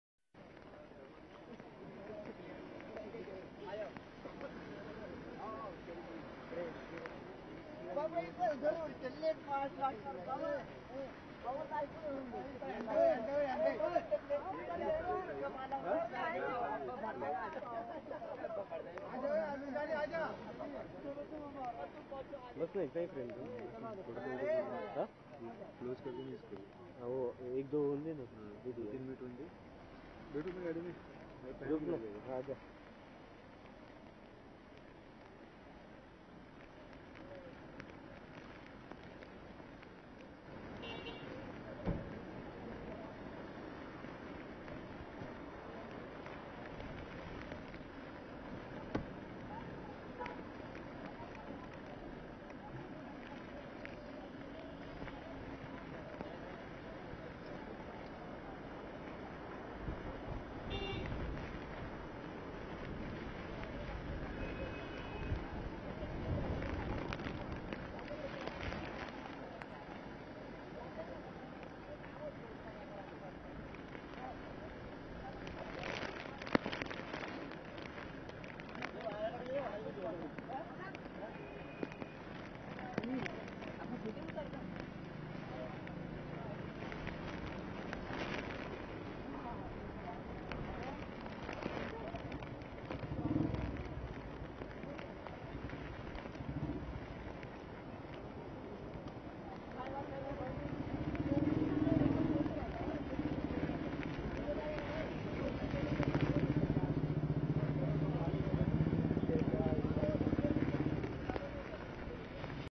Heavy Rainfall Predicted in Mumbai in Next 24 Hours
Heavy rains lash Mumbai, India heavy to very heavy rainfall predicted in over next 24 hrs.